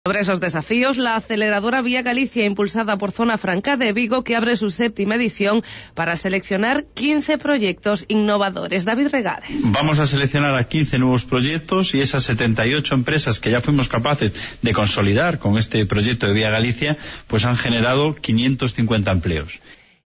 VIAGALICIA | Declaraciones del delegado de ZFV, David Regades, en Cadena SER
📻 HxH | David Regades, delegado de la ZonaFrancaVigo, en Grupo RadioVigo sobre ViaGalicia Aceleradora "Vamos a seleccionar 15 nuevos proyectos. Las 78 empresas que ya han participado en ViaGalicia han generado 550 empleos"